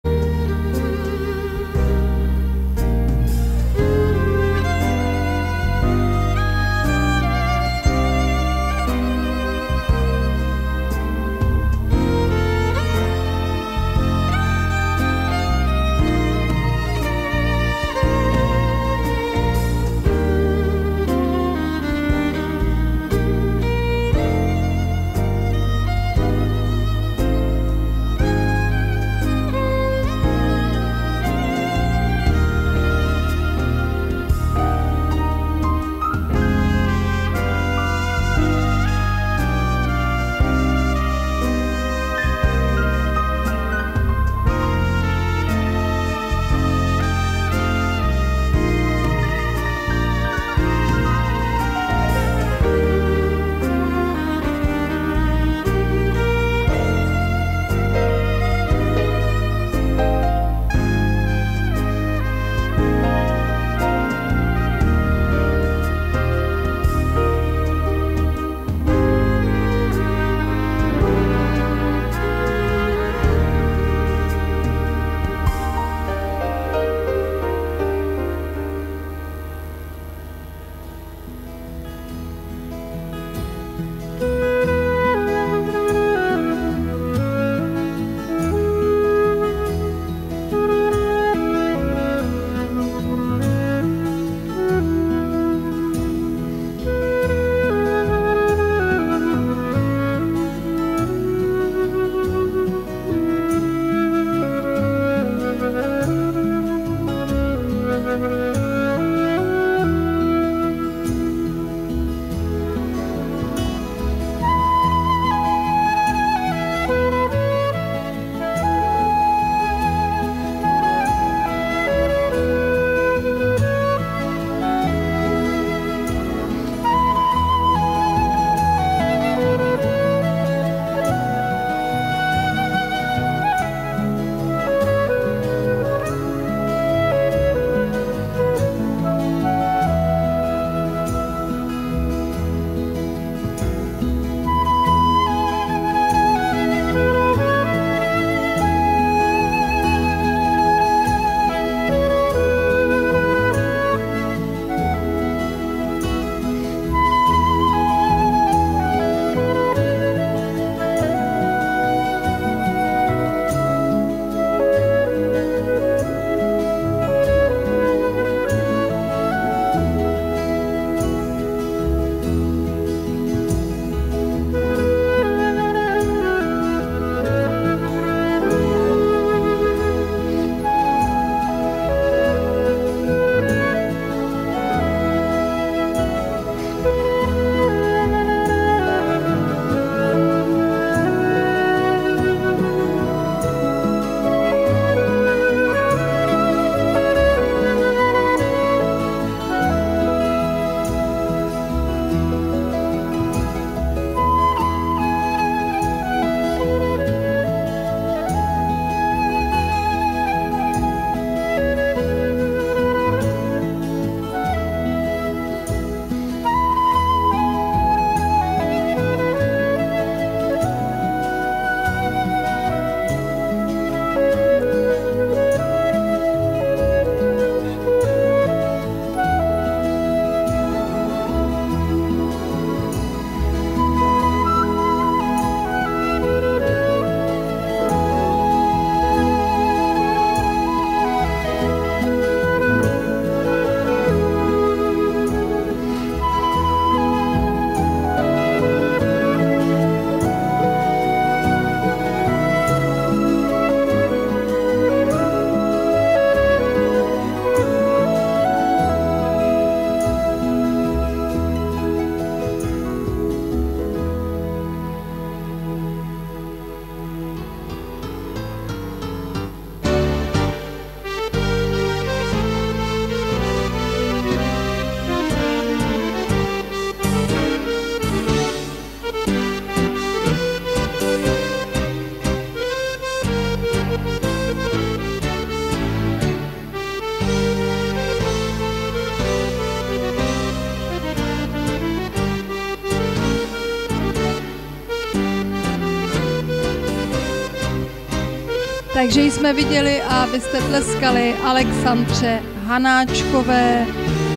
MČR 2013 - 28.7. - Alexandra Hanáčková / Don Dimanche - DJ/2009
MČR drezura - Hradištko u Sadské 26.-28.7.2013 / Czech dressage championship 2013